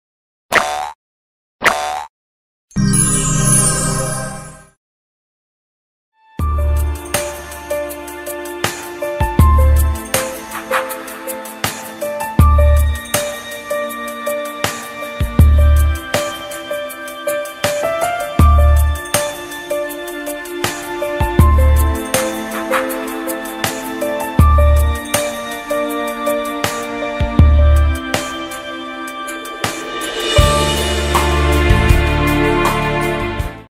Сравнение Milesight H 264 и H 265+
Качество видеосъемки H 264 и  H 265+ в сравнении